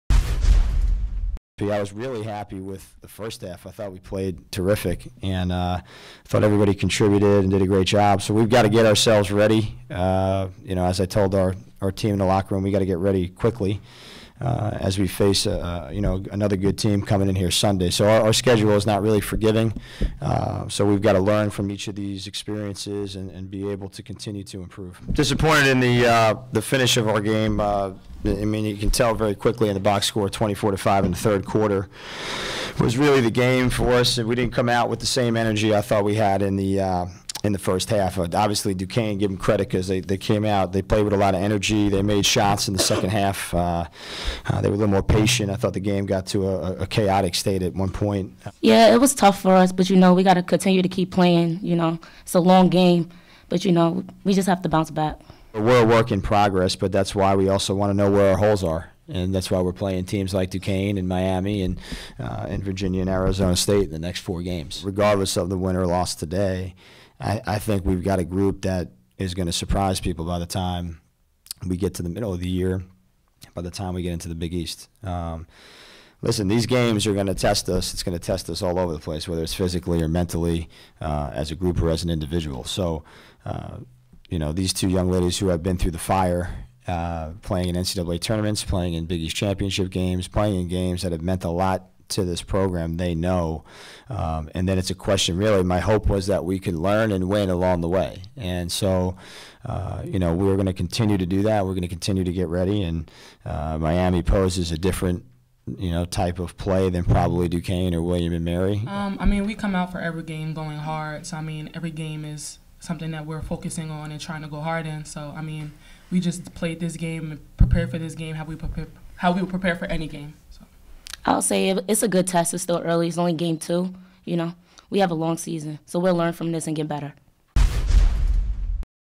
Recap: Duquesne 71, St. John's 66
Despite getting 17 points from Aaliyah Lewis and 16 from Jade Walker, the St. John's women’s basketball team dropped a 71-65 decision to Duquesne at Carnesecca Arena Thursday night.